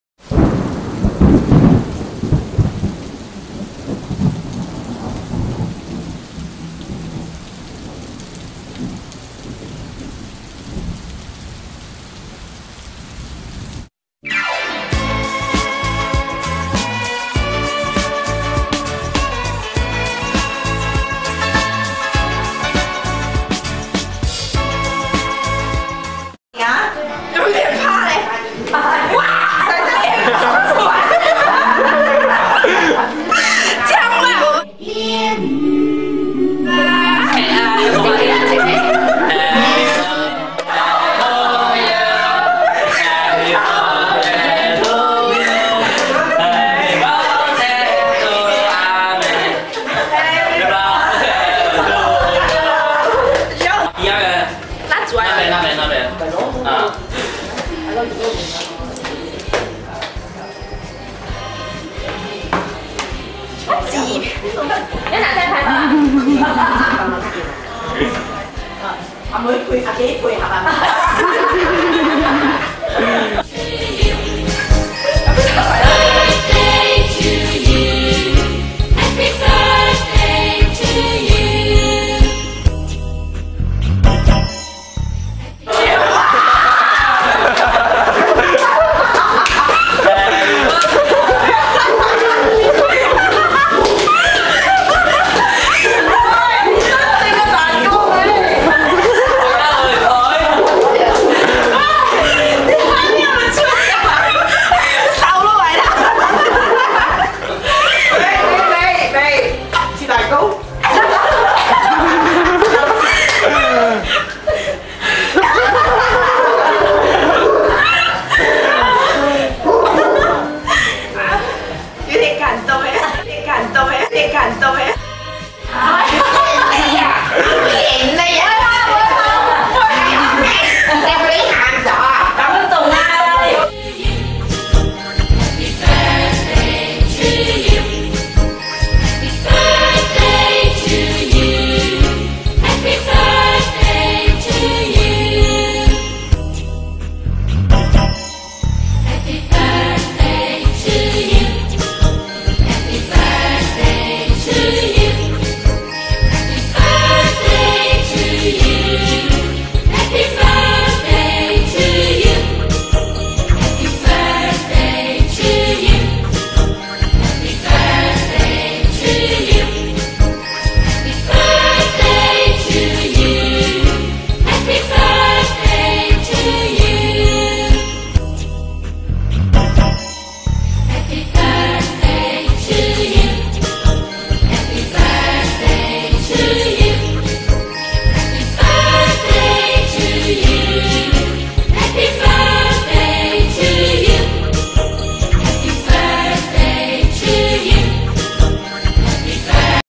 Chin Mei's Birthday+ her 真情流露篇。。
Happy birthday to our LOVELY MEI MEI. We hope that u like your 燒肉 birthday cake :)